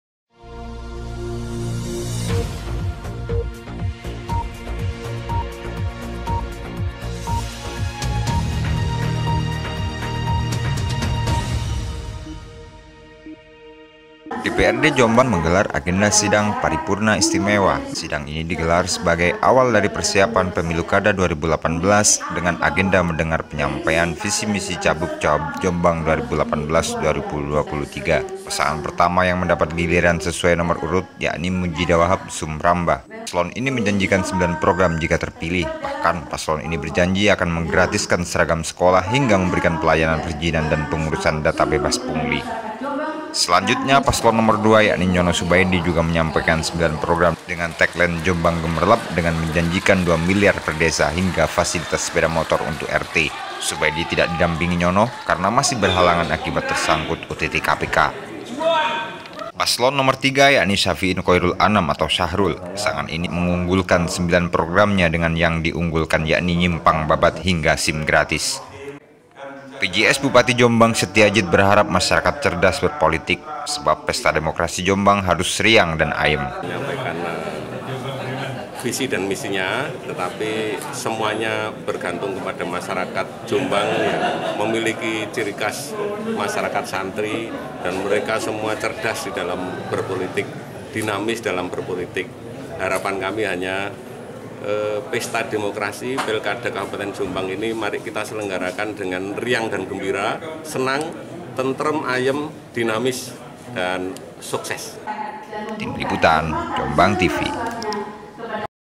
Paripurna Istimewa Visi Misi Cabup- Cawabup Pilkada Jombang
JOMBANGTV.COM - DPRD Jombang, Kamis (15/2) kemarin menggelar  agenda sidang paripurna istimewa. sidang ini digelar sebagai awal dari persiapan pemilu kada 2018 dengan agenda mendengar penyampaian visi-misi Cabup-Cawabup Jombang 2018 – 2023.
Pasangan pertama yang mendapat giliran sesuai nomor urut yakni, Mundjidah Wahab - Sumrambah. Paslon ini menjanjikan 9 program  jika terpilih. Bahka paslon ini berjanji akan menggratiskan seragam sekolah hingga memberikan pelayanan perijinan dan pengurusan data bebas pungli.
Selanjutnya, paslon nomor dua yakni Nyono-Subaidi, juga menyampaikan 9  program dengan tagline Jombang Gemerlap dengan menjanjikan 2 milyar perdesa hingga fasilitas sepeda motor untuk RT. Subaidi tidak didampingi Nyono, karena masih berhalangan akibat tersangkut OTT KPK.
Paslon nomor 3 yakni, Syafi’in-Khoirul Anam atau Syahrul, Pasangan ini sempat mengkiritisi program dua milyar untuk desa karena program tersebut merupakan program pusat siapapun yang akan jadi. Meski demikian, Syafiin sempat mengunggulkan 9 programnya, dengan yang di unggulkan, yakni Nyimpang Babat,  hingga SIM Gratis.
PJS Bupati Jombang, Setiadjit berharap masyarakat cerdas berpolitik. sebab, pesta demokrasi jombang harus riang dan ayem.(rul)